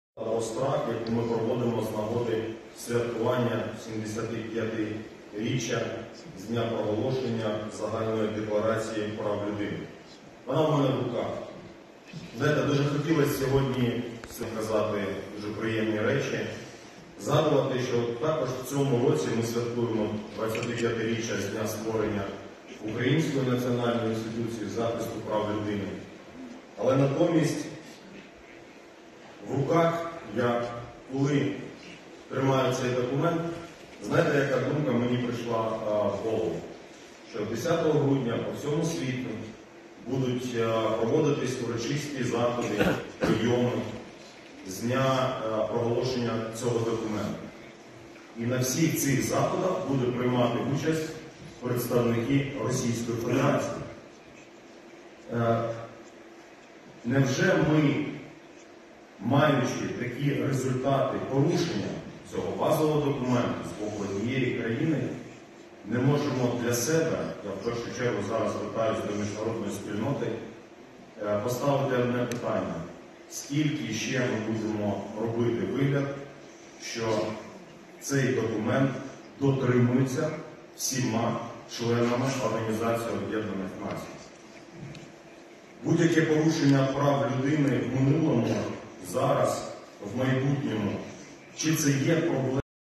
Обраќање на украинскиот омбудсман Лубинец на Конференција за човекови права „Слобода или страв“
Украинскиот омбудсман Дмитро Лубинец зборуваше во контекст на загубите од руската агресија врз Украина најголеми жртви се децата.
„Ако вратиме по едно дете на ден, ќе ни бидат потребни 55 години да ги вратиме сите деца што се киднапирани од окупираните делови на Украина“, истакна Лубинец.